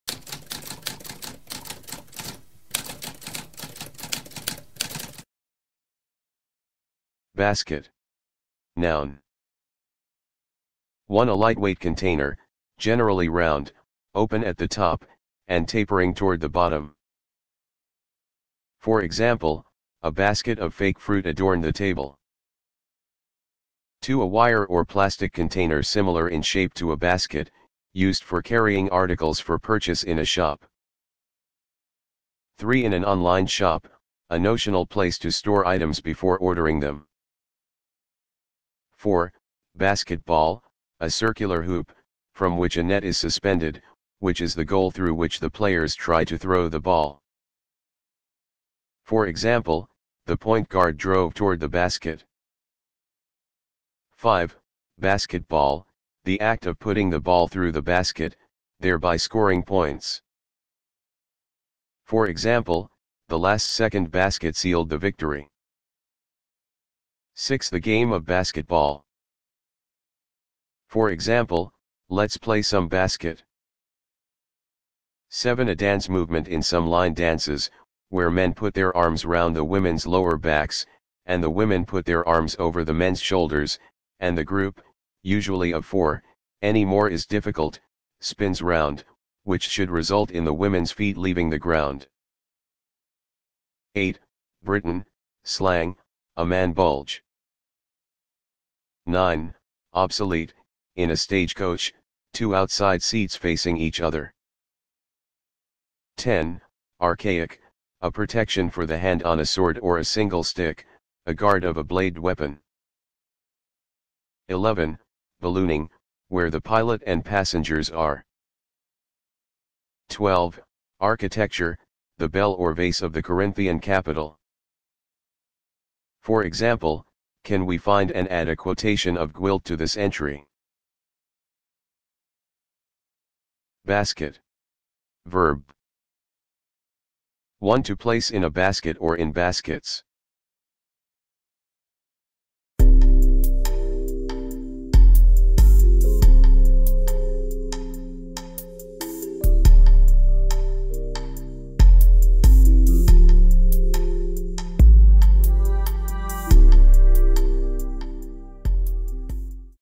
What does basket mean?
A spoken definition of basket.

Intro Sound:
Typewriter - Tamskp